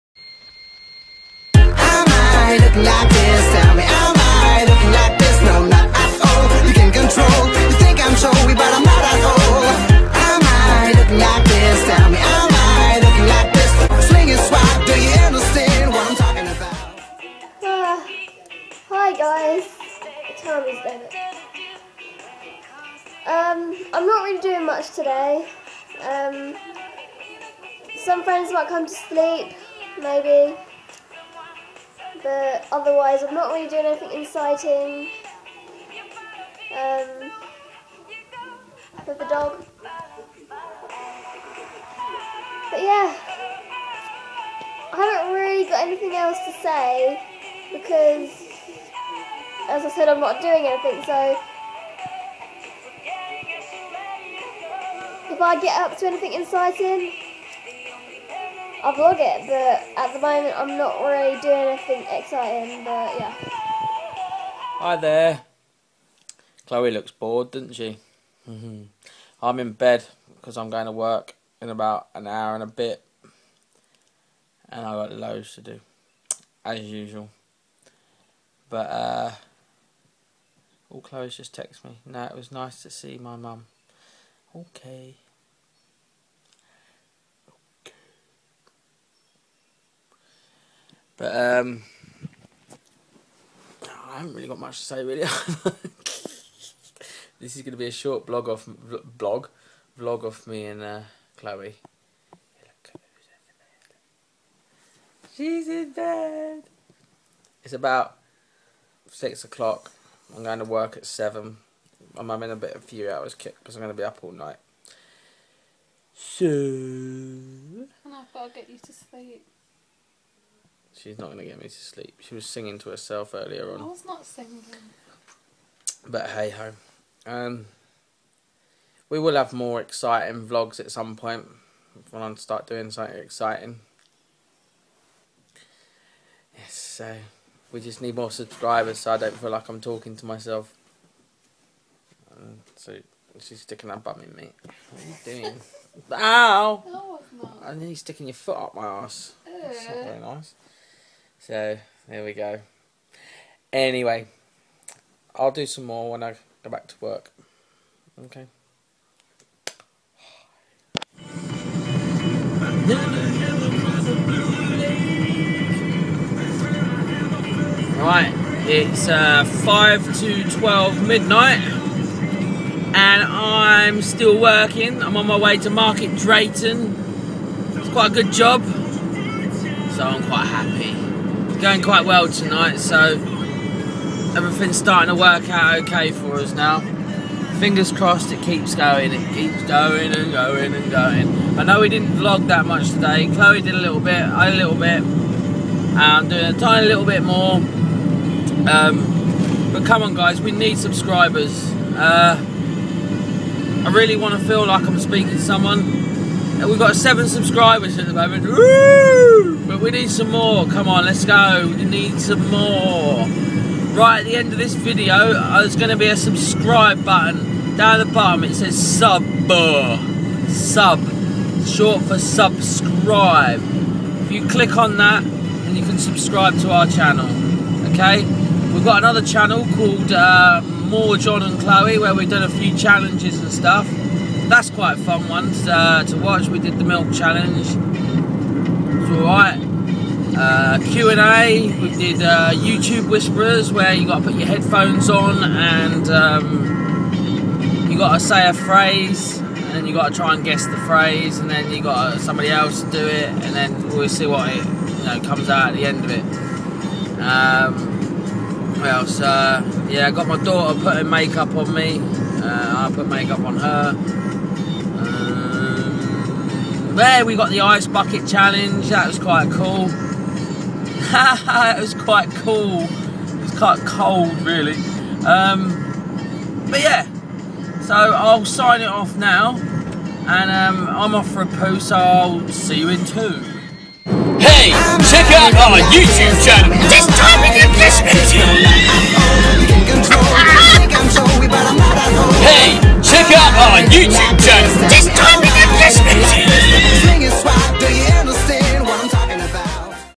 Vlogging from the south and the middle of England
Father and daughter vlogging, my daughter lives south of England, and I'm in the midlands but we have a weekend every 3rd weekend together. But still vlog every day.